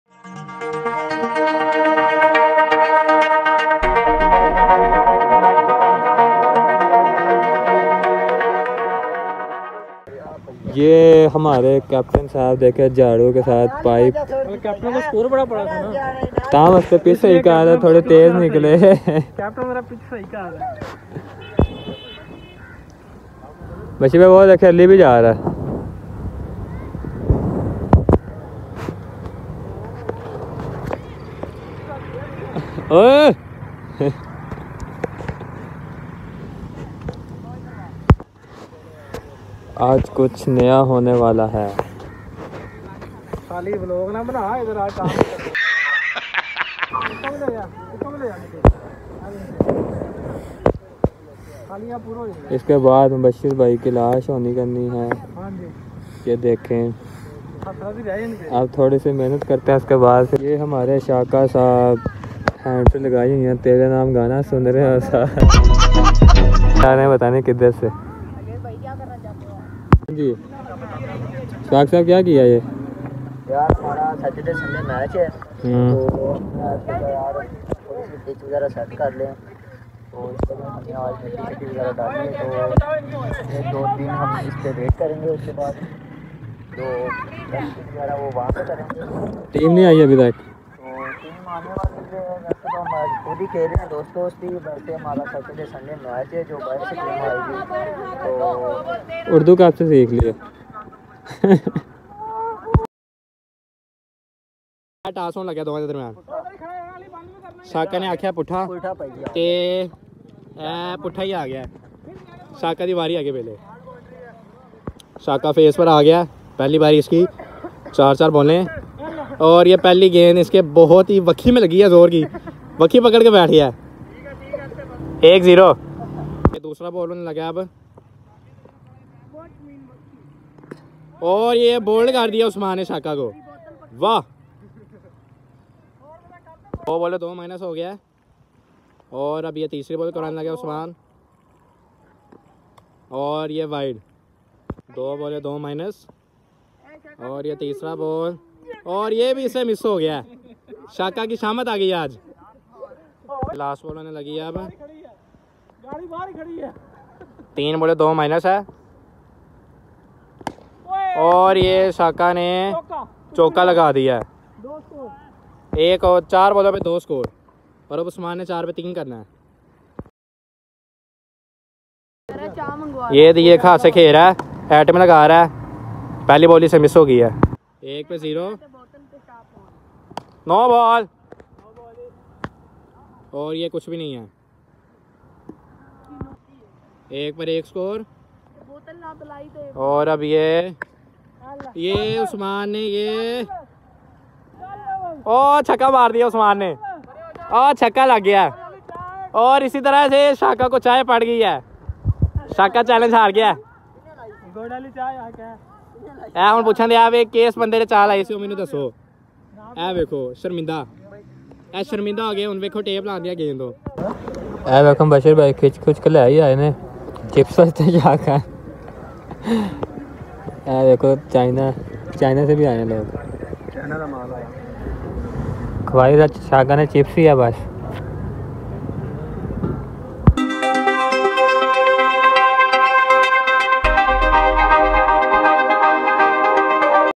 One hand cricket || shaka aj har gya🥹🥹
Shaka to aj phr sy har gya
2 hatho sy khyla  All boundaries pr or one hand challenge har gya
Chips khilaye wa😱😱😱